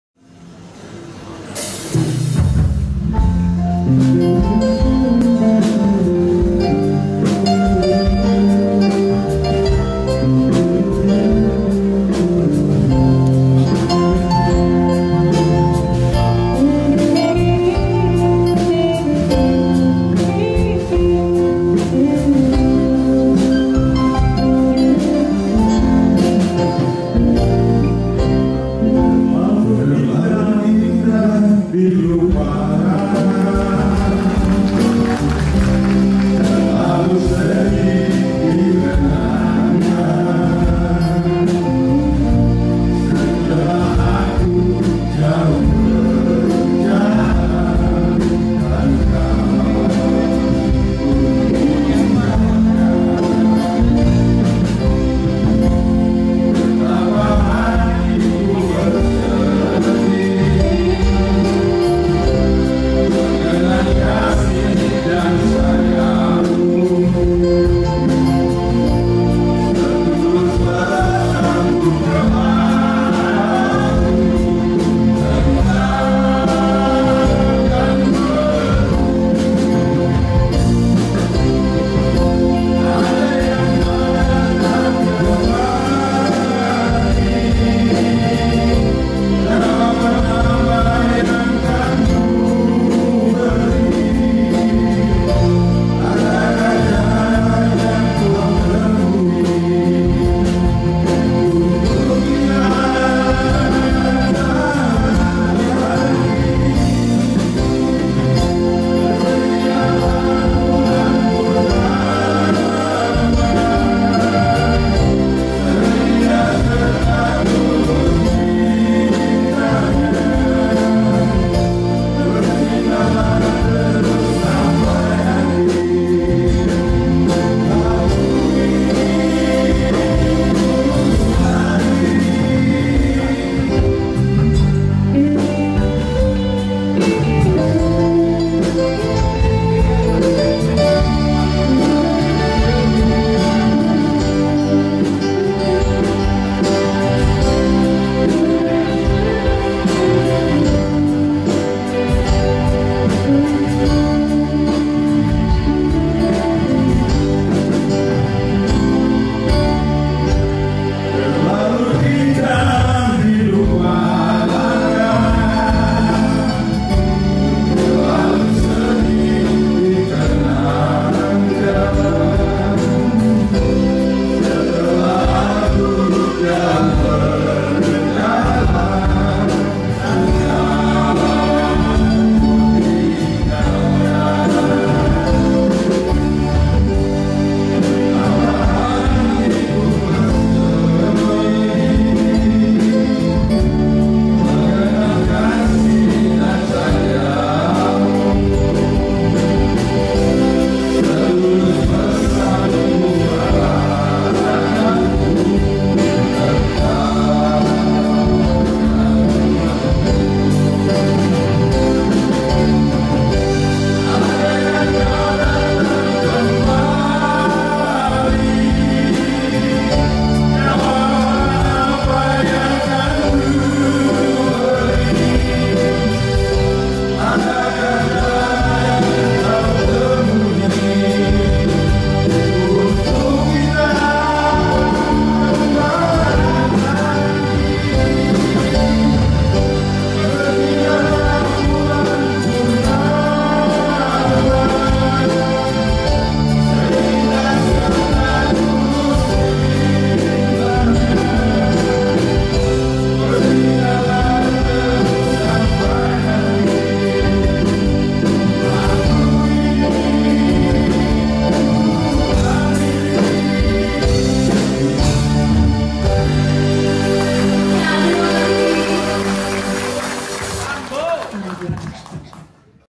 GM PT.PJB (Pembangkitan Jawa Bali ) Bernyanyi Bersama
Pihak manajemen PT.PJB  (Pembangkitan Jawa Bali ) melakukan bernyanyi bersama-sama